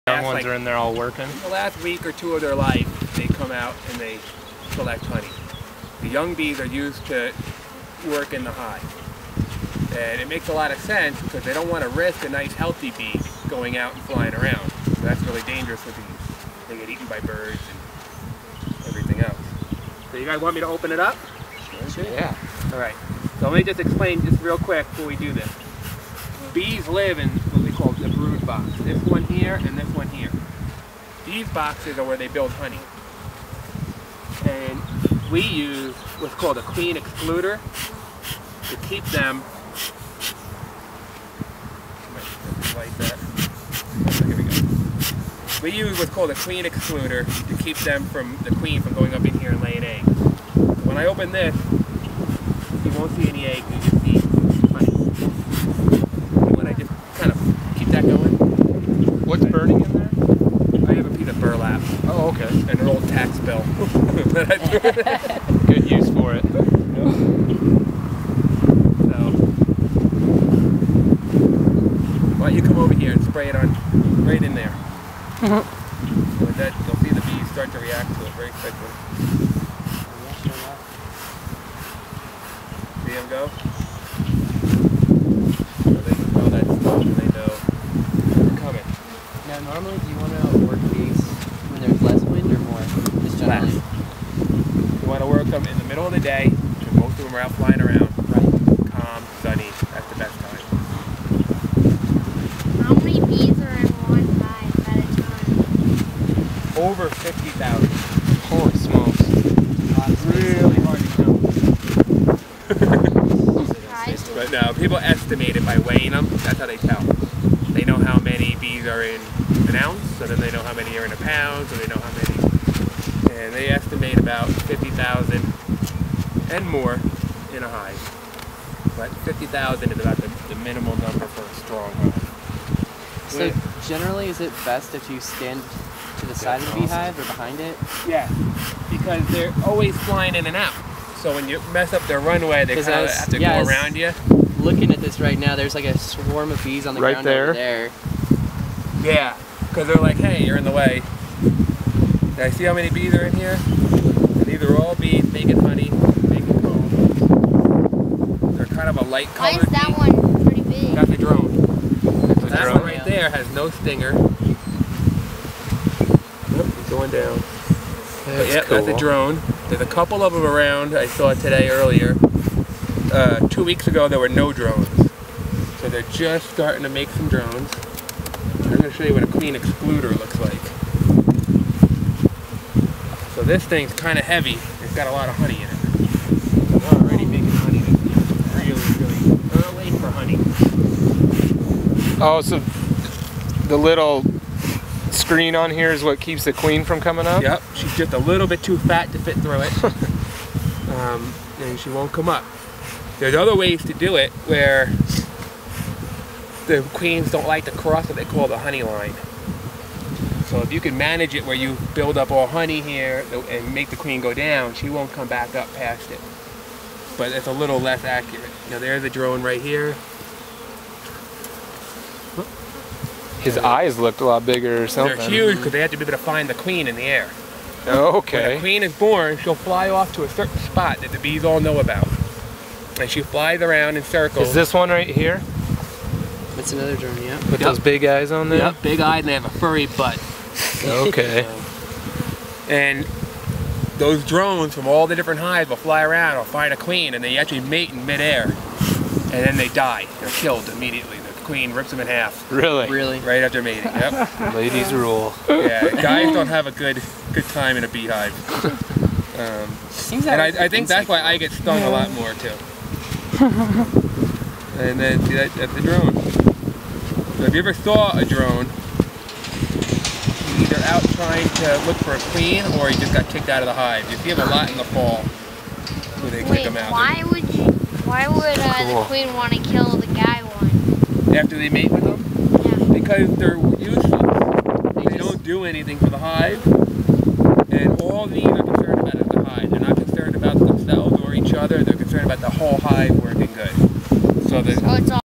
Bee Hives at Gulley Greenhouse
Front Range Community College bee experts came to teach us about bees at Gulley Greenhouse on Earth Day!  Here is our trip to the hives on Gulley property.